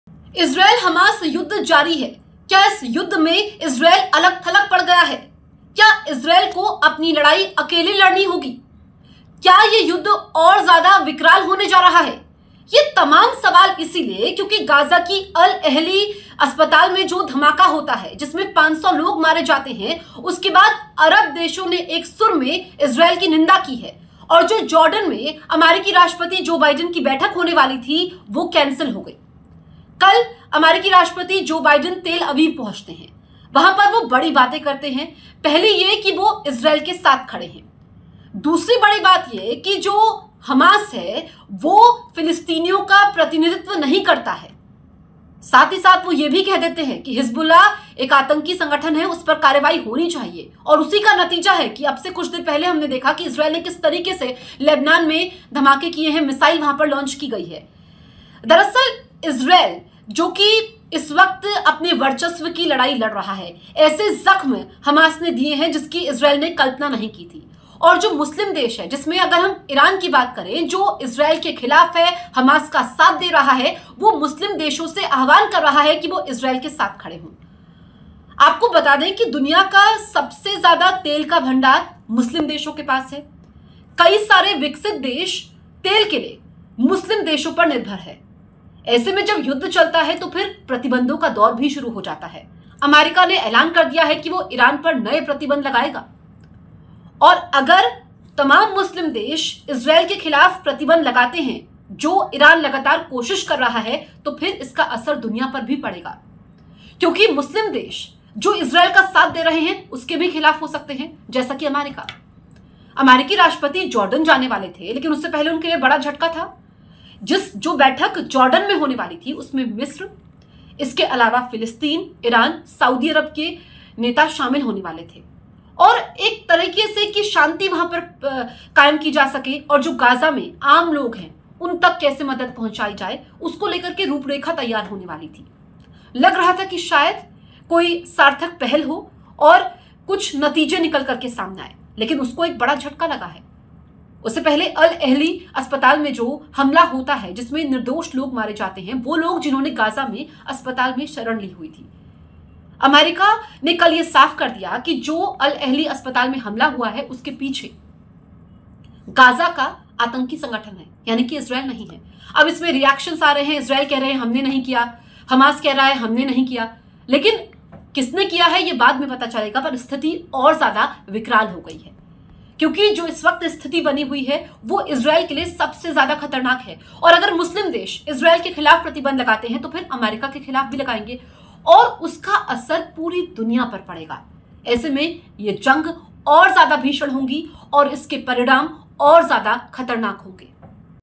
Israel Hamas battle : इजराइल पहुंचकर Joe Biden ने की बड़ी बात| Al-Ahli Hospital | Benjamin Netanyahu
इजरायल और हमास के बीच जंग के 12वें दिन भी एयरस्ट्राइक, बमबारी, रॉकेट और सायरन की आवाजें गूंजती रहीं. इस युद्ध में अब तक 4,200 से अधिक लोगों की जानें जा चुकी हैं...